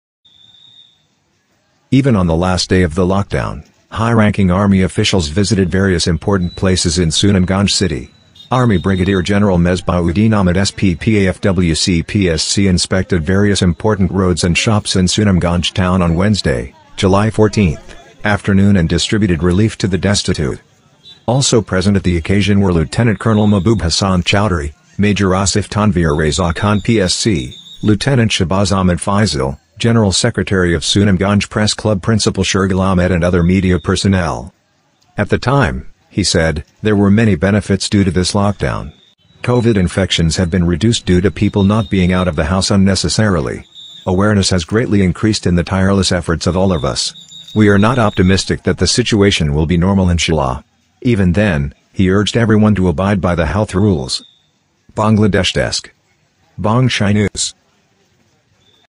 Last Day Of The Lockdown : High Ranking Army Officers Were On The Ground
Even on the last day of the lockdown, high-ranking army officials visited various important places in Sunamganj city.
Army Brigadier General Mesbah Uddin Ahmed SPP AFWC PSC inspected various important roads and shops in Sunamganj town on Wednesday (July 14) afternoon and distributed relief to the destitute.

Also present at the occasion were Lt. Col. Mahbub Hasan Chowdhury, Major Asif Tanvir Reza Khan PSC, Lt. Shahbaz Ahmed Faisal, General Secretary of Sunamganj Press Club Principal Shergul Ahmed and other media personnel.

At the time, he said, there were many benefits due to this lockdown. Covid infections have been reduced due to people not being out of the house unnecessarily. Awareness has greatly increased in the tireless efforts of all of us. We are not optimistic that the situation will be normal inshallah. Even then, he urged everyone to abide by the health rules.

Bangladesh Desk
Bongshai News.